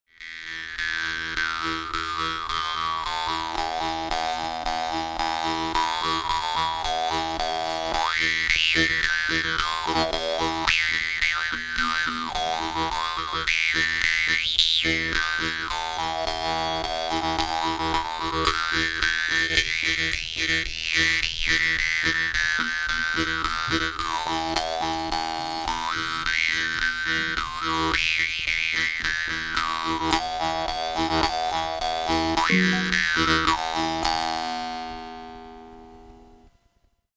Russian jew's harp By Glazyrin - Mongolians
Beautiful harp with engraving. The sound is medium-high, quite sonorous, suitable for fast playing. A good, interesting Jew's harp for both beginners and continuing players on the Jew's harp